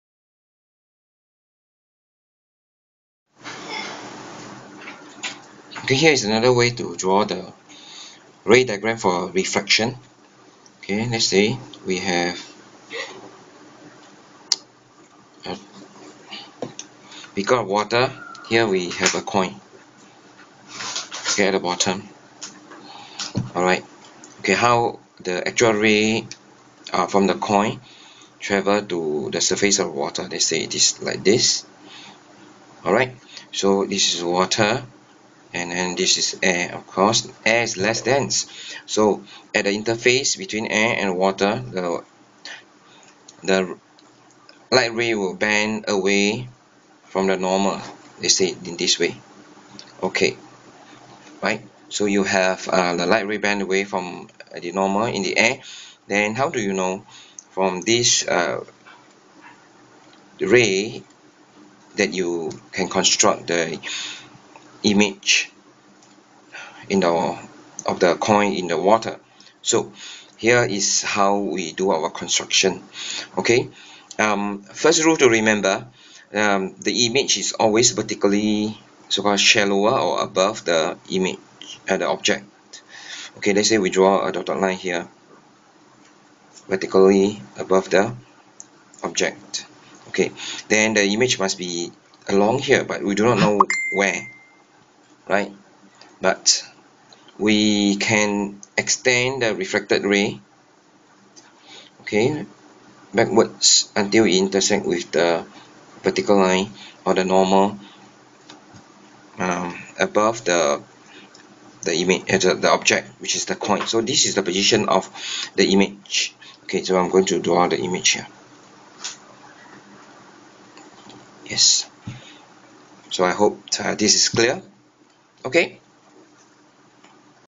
Refraction ray diagram - object in water
This video plainly show how to construct a simple ray diagram of refraction of light for an object in a beaker of water.